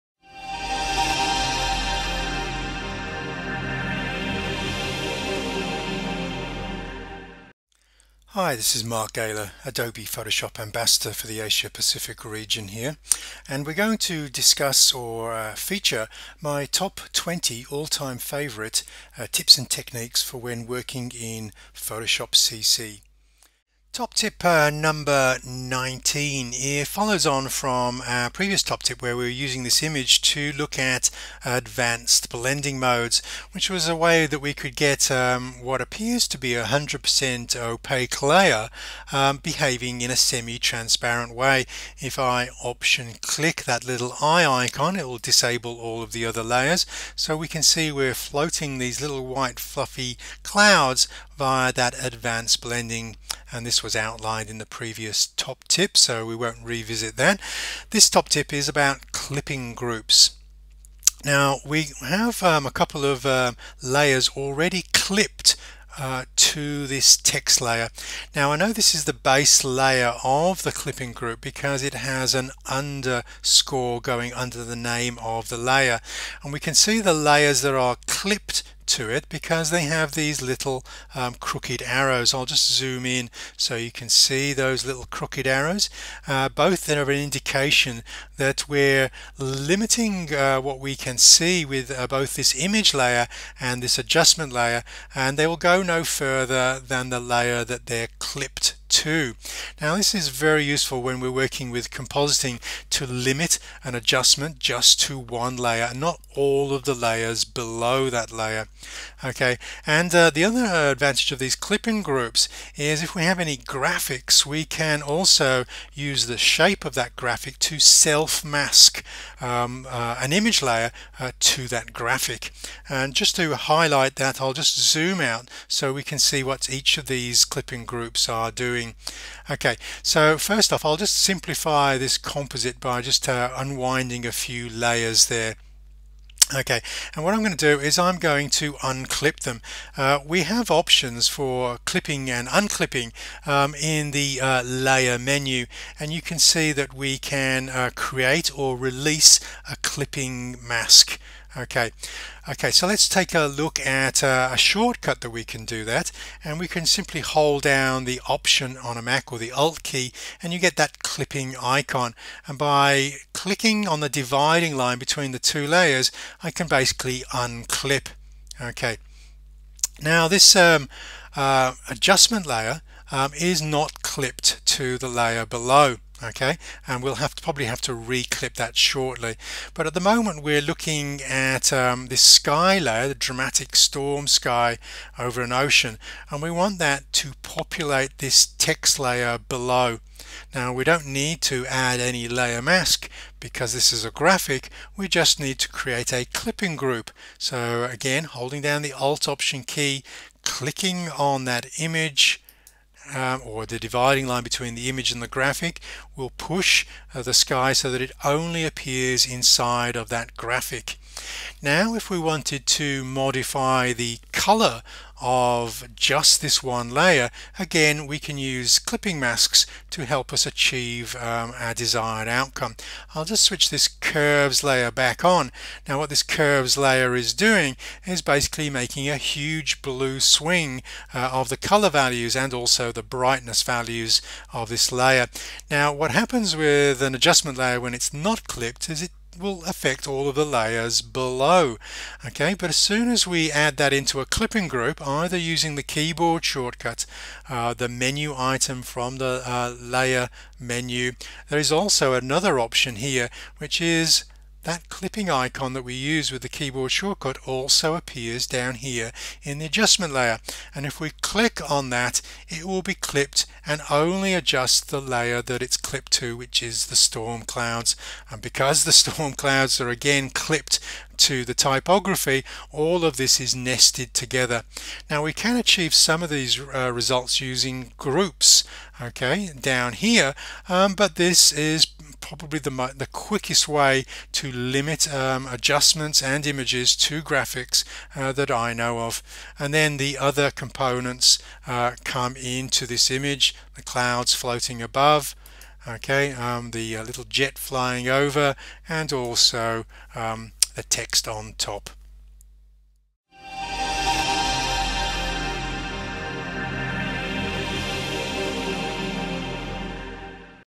Top Tip 19 - Photoshop CC Tips & Techniques - Clipping Masks
Photoshop CC Top 20 Tips & Techniques

Mark' Galer's Top Twenty Tips and Techniques to help you master Photoshop CC
Learn how to edit and process your photos with confidence using simple and quick processing techniques.

This series of tutorial movies have been created to help educate photographers how to organise (Catalog), select and process (Develop) their photographic images when using Adobe Photo Editing post-processing software.

Mark Galer is a photographer, educator and Sony Imaging Ambassador.